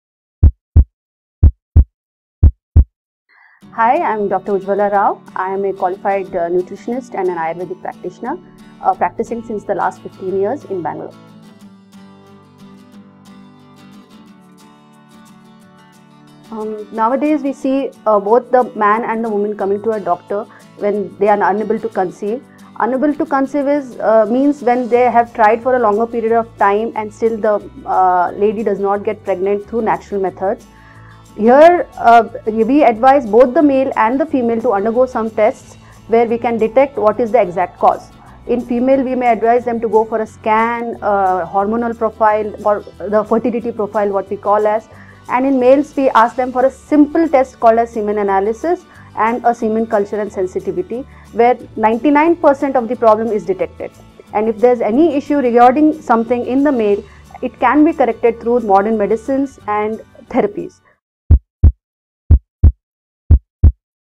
Infertility tests for men and women - Dr. Ujwala Rao
Now a day&rsquo;s both the men and women coming to the doctor and they are complaining that unable to conceive even after trying for a long period. So at that point of time both of you need to undergo infertility test. For female we may advise you to go for scan, hormonal profile and fertility profile. For male semen analysis. By doing this 99% of problem should be detected.